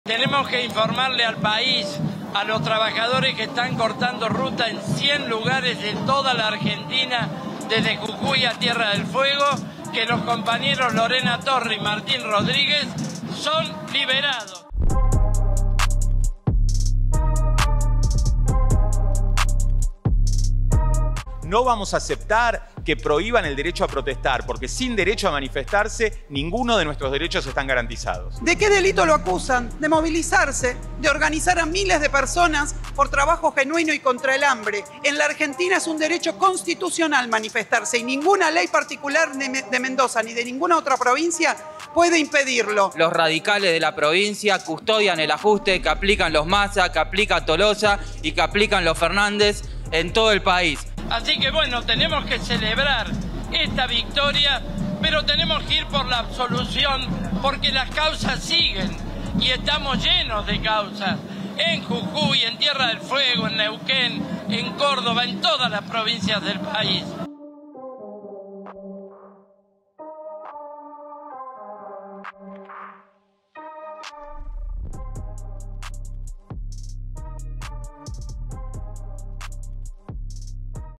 Triunfo popular: Liberaron a Lorena y Martín // Basta de presos por luchar
Mientras miles cortaban los accesos a la capital y 100 puntos estratégicos en todo el país, ya concentración permanecía frente al Polo Judicial en Mendoza, la justicia ordenó liberar a Lorena Torres y Martín Rodríguez, militantes del Polo Obrero y el Partido Obrero que se encontraban presos políticos del gobierno radical de Rodolfo Suárez. Ambos se encontraban detenidos desde el 14 de marzo, cuando se los encarceló mientras participaban de la movilización de la Unidad Piquetera, en reclamo de trabajo genuino y alimento para los comedores populares.